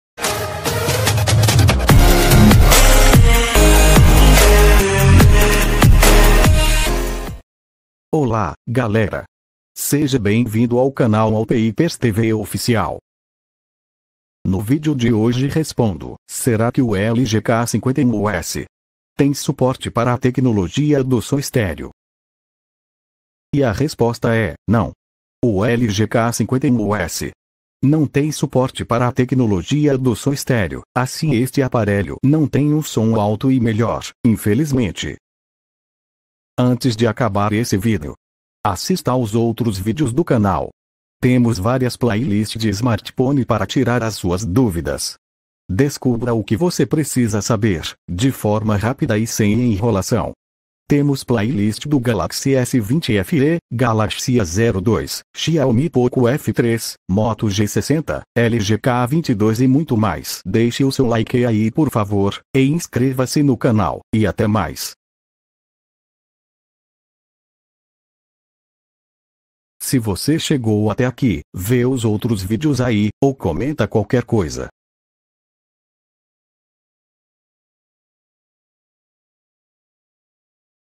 🔴 O LG K51S TEM SOM ESTÉREO?
No vídeo de hoje respondo se o LG K51S tem suporte para a tecnologia do som estéreo, e a resposta é não. O LG K51S não tem suporte para a tecnologia do som estéreo, assim este aparelho não tem um som alto e melhor, infelizmente.